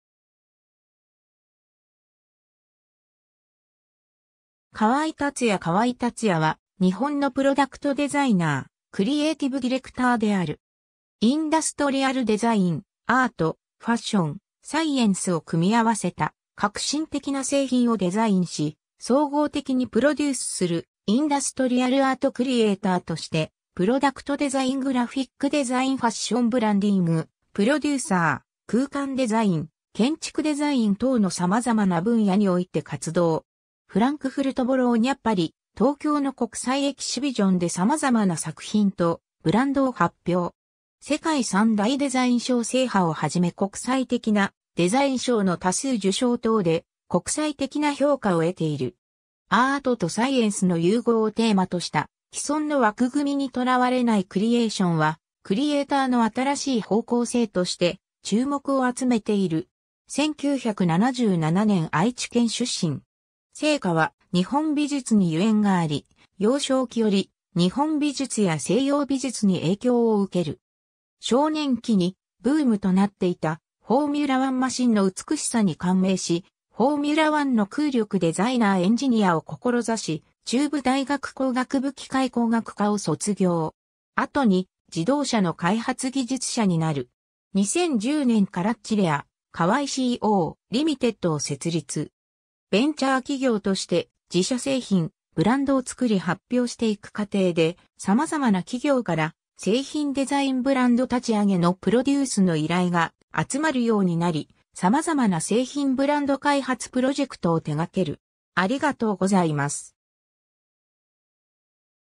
川合辰弥
川合 辰弥 川合 辰弥（かわい たつや、1977年4月2日-）は日本のプロダクトデザイナー・クリエイティブディレクターである。
 インダストリアルデザイン、アート、ファッション、サイエンスを組み合わせた、革新的な製品をデザインし総合的にプロデュースする「インダストリアルアートクリエイター」として、プロダクトデザイン・グラフィックデザイン・ファッション・ブランディングプロデューサー・空間デザイン・建築デザイン等の様々な分野において活動。
フランクフルト・ボローニャ・パリ・東京の国際エキシビジョンで様々な作品とブランドを発表。
世界三大デザイン賞制覇をはじめ国際的なデザイン賞の多数受賞等で国際的な評価を得ている。
アートとサイエンスの融合をテーマとした既存の枠組みにとらわれないクリエイションは、クリエイターの新しい方向性として注目を集めている。
 1977年愛知県出身。
生家は日本美術に由縁があり、幼少期より日本美術や西洋美術に影響を受ける。
少年期にブームとなっていたフォーミュラ・ワン マシンの美しさに感銘しフォーミュラ・ワンの空力デザイナー・エンジニアを志し中部大学 工学部 機械工学科を卒業。
後に自動車の開発技術者になる。
 2010年 Carozzeria Kawai Co.Ltd を設立。
ベンチャー企業として自社製品・ブランドを創り発表していく過程で、様々な企業から製品デザイン・ブランド立ち上げのプロデュースの依頼が集まるようになり、様々な製品ブランド開発プロジェクトを手がける。